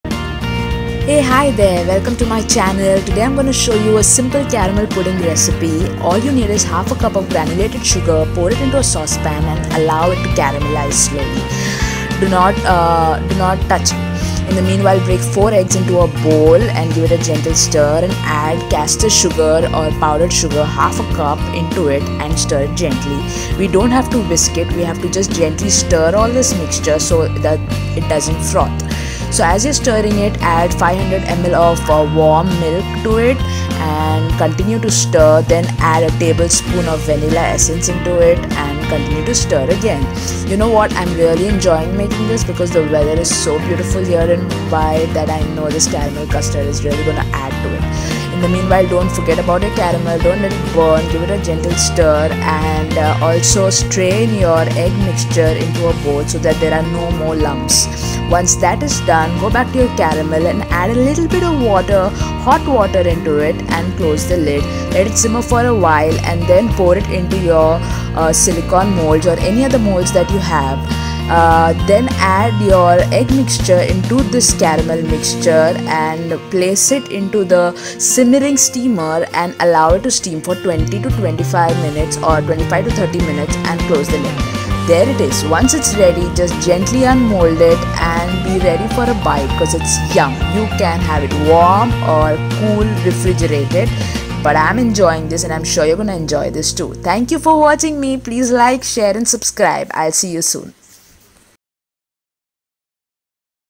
Caramel Pudding ( easy, yummy, quick and silky smooth)Try it.
Well I always thought that caramel pudding is quite tough and messy and eggyy. But to our surprise I found out that it was one of the easiest desserts we could make on our daily basis to satisfy sweet cravings and also to celebrate togetherness. Just a few simple steps and You have your very own silky, smooth caramel glazed pudding. So sit back, enjoy this weather and watch it and make it yourself.